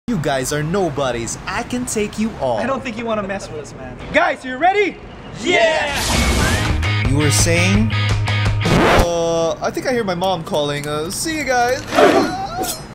When You Think Your Cosplay Is The Best (Power Rangers)
Cosplayer respect is important. If you work hard in making your own costume, that deserves respect. But this is just too funny!